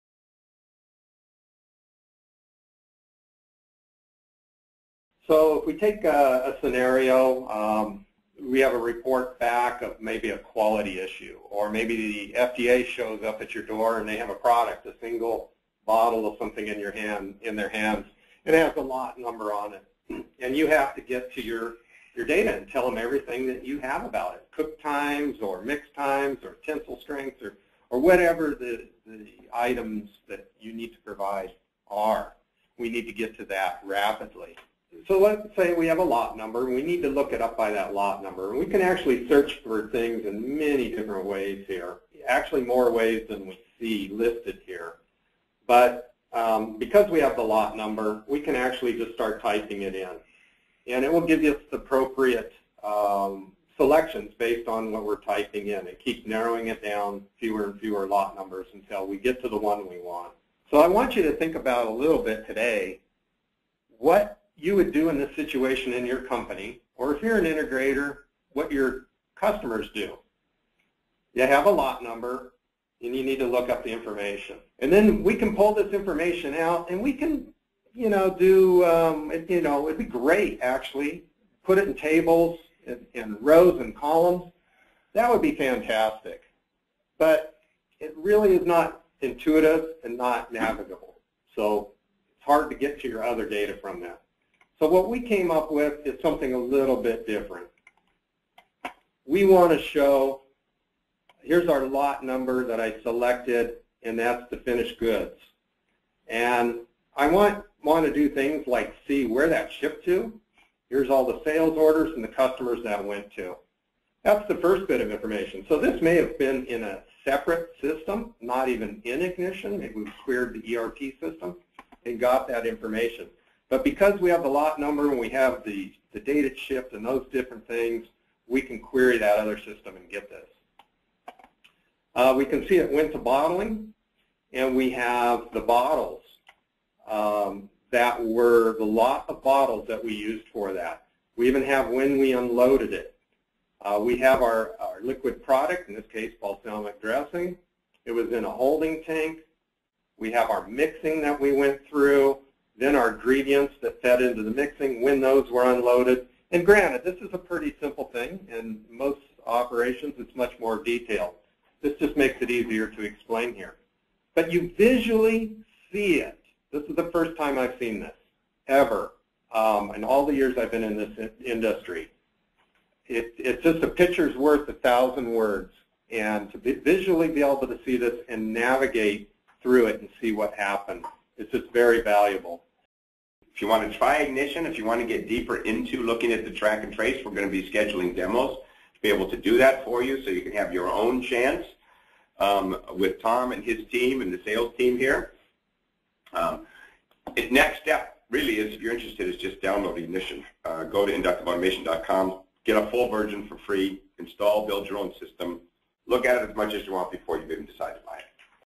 Visualizing the Manufacturing Process - Track & Trace
As technology evolves and regulatory demands significantly increase, your enterprise not only needs a track and trace system, it needs one that is fully integrated with SCADA, MES, ERP and other systems. The Ignition Track and Trace Module brings information from a wide variety of sources into one interface that shows exactly where any product came from and where it currently is during the manufacturing process.

Take a tour through the newest Ignition module at this free webcast. Join us to learn more about:

• How the module brings OEE, SPC, recipe, historical trend and more information together
• How easy it is to track products from start to finish using the module
• Why expanded food traceability requirements will affect many companies
• Why pharmaceutical manufacturers need to ramp up to unit-level traceability in the near future
• Querying data from systems outside of Ignition
• How Track and Trace helps significantly reduce risks posed by product recalls, regulatory changes and human error